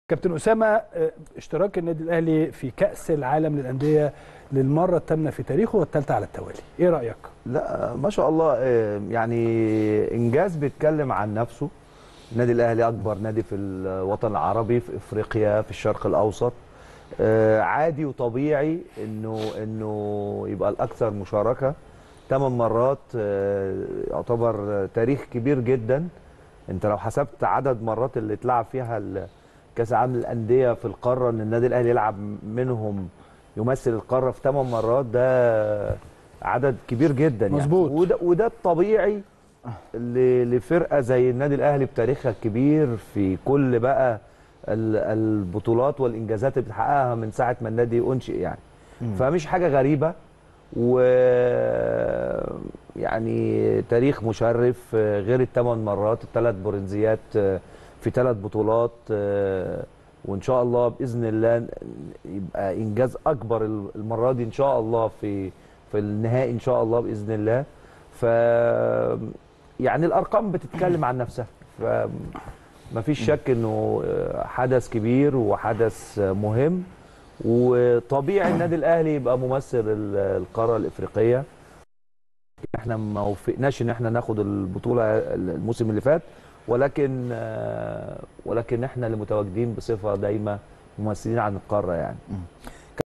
عرابي: تواجد الأهلي للمرة 3 على التوالي و8 في تاريخه بمونديال الأندية انجاز وشرف للرياضة المصرية
أسامة عرابي: تواجد الأهلي للمرة الثالثة على التوالي والثامنة في تاريخه بمونديال الأندية انجاز وشرف كبير للرياضة المصرية